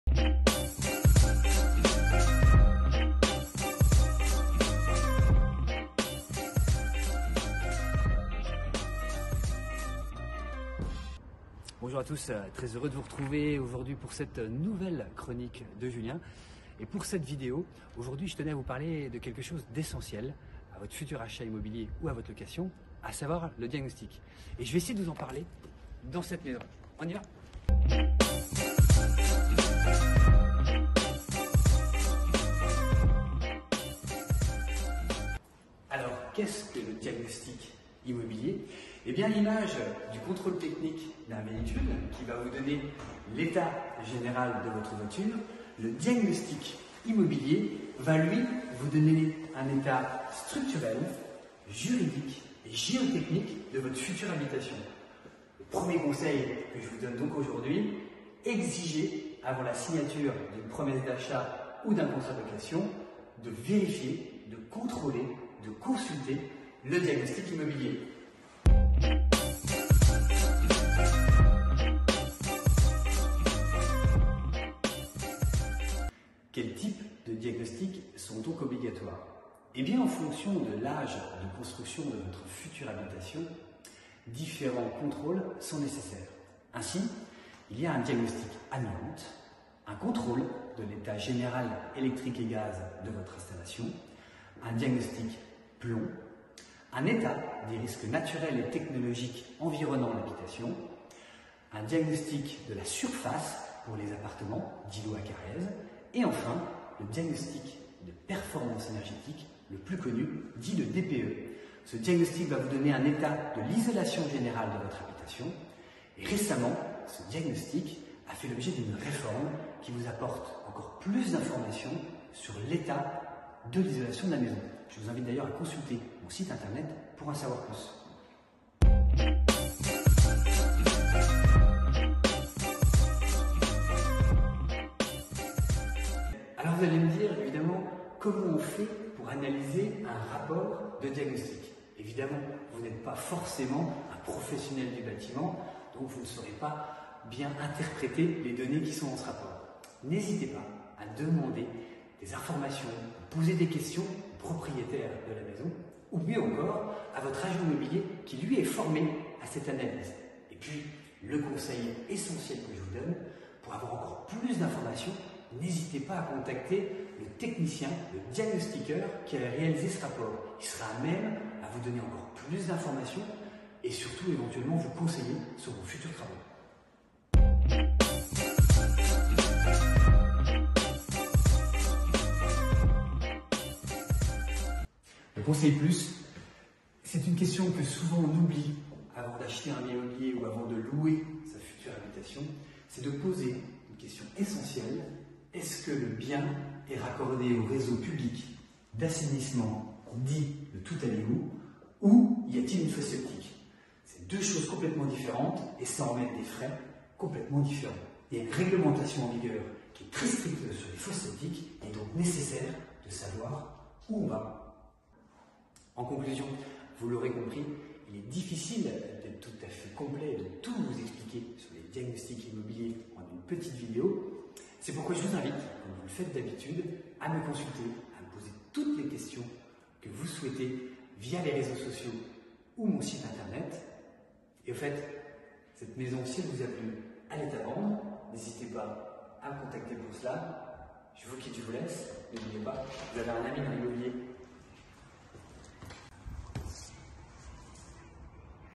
Les chroniques de Julien #2 - Les diagnostics immobiliers
Pour cette 2e vidéo, nous faisons le point et vous donnons des conseils sur les diagnostics immobiliers dans le cas d'une vente ou d'une location